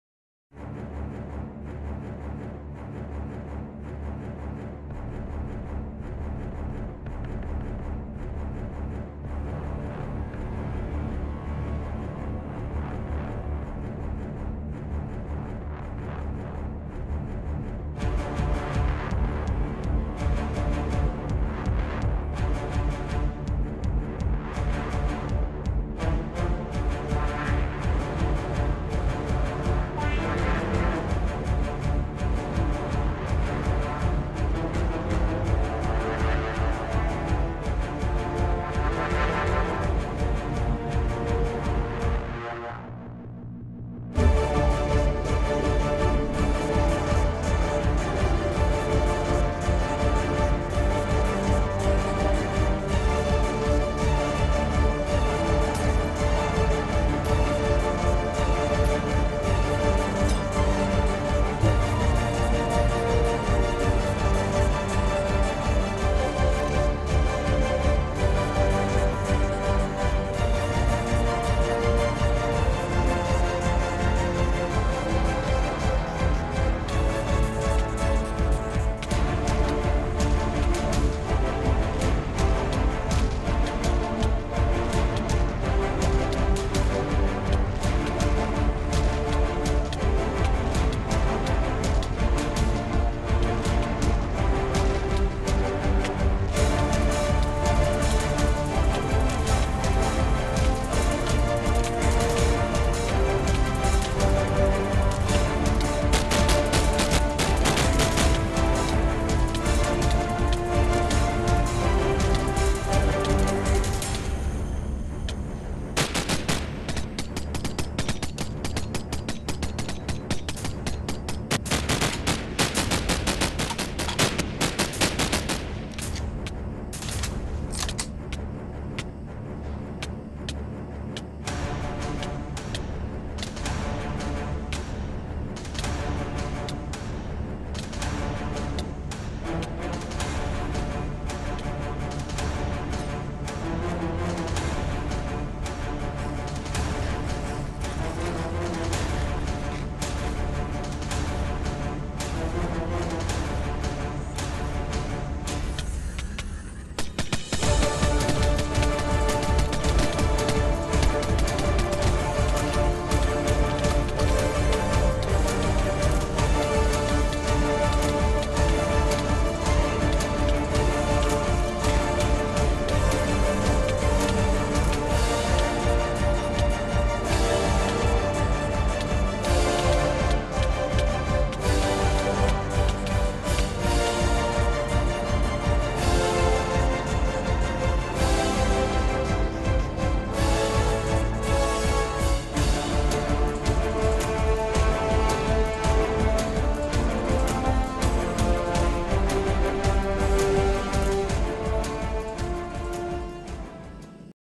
Deathcraft: Crack Pack #13.5 - Battle at Bedrock! Brink Murdered!!
With the kit we all have these days, these battles are starting to take some time ... :P

SPOILERS: sneak preview of stuff to come ...

A like or comment is always amazing and helps out more than you know :)